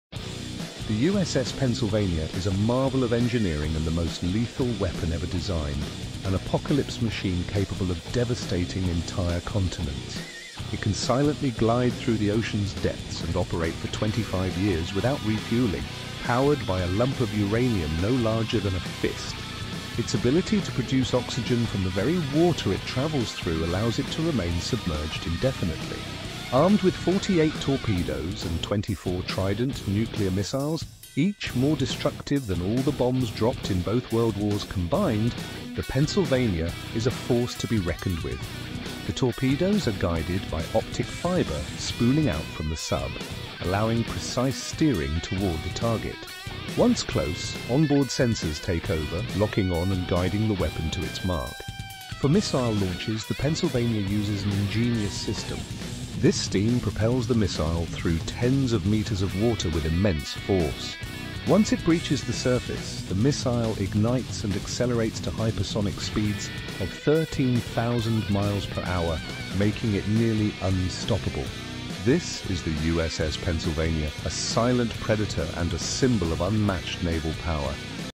Meet The USS Pennsylvania: The Ultimate Powerhouse Submarine!
Join us as we explore the USS Pennsylvania, a behemoth of the deep that's touted as one of the most powerful submarines ever built. With its impressive arsenal of Trident II missiles and unparalleled stealth capabilities, this Ohio-class submarine is a force to be reckoned with. But what makes the USS Pennsylvania so formidable? From its advanced sensors and communication systems to its impressive speed and maneuverability, we'll delve into the features that set it apart from other submarines. Whether you're a military enthusiast or just curious about the world of underwater warfare, this video is for you. So, dive in and discover the incredible capabilities of the USS Pennsylvania, a true marvel of modern naval engineering.